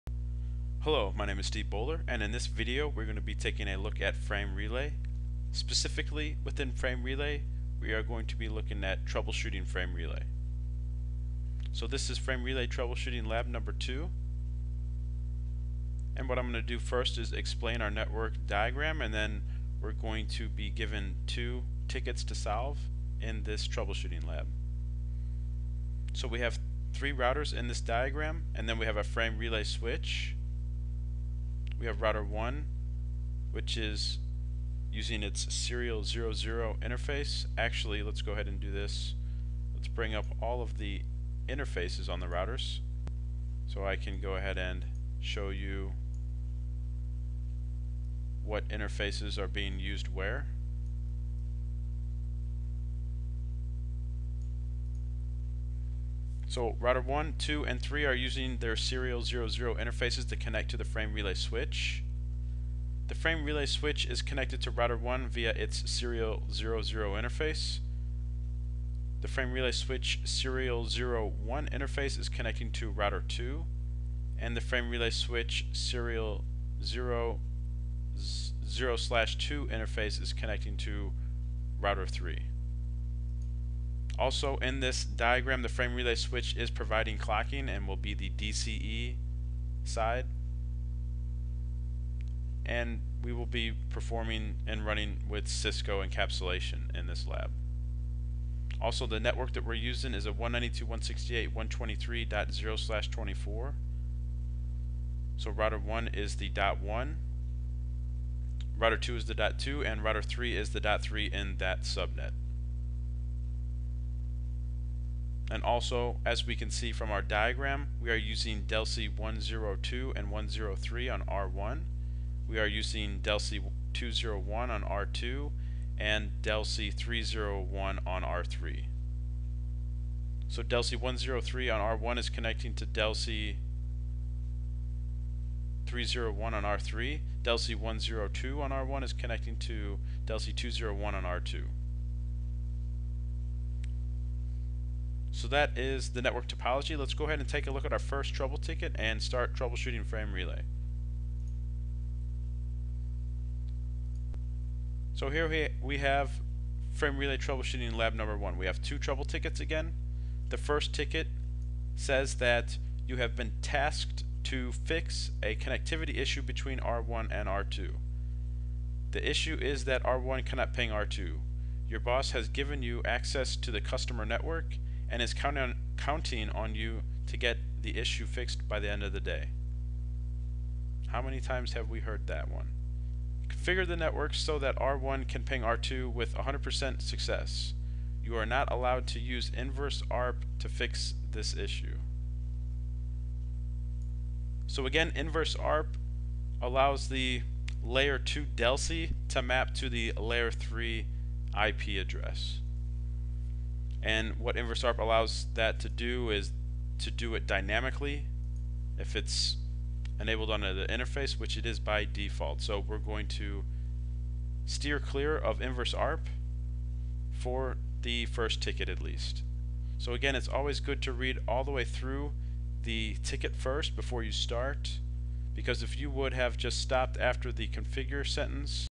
Frame Relay Troubleshooting Lab 2 Part 1 Trailer
Download Cisco R&S Series 1 and 2 instantly from the link below and save 25%

In this video you are presented with complex issues in which are broken down into trouble tickets or tasks. I walk you through how to troubleshoot using CCIE level skills to restore network connectivity and come up with a valid solution to the trouble tickets or tasks.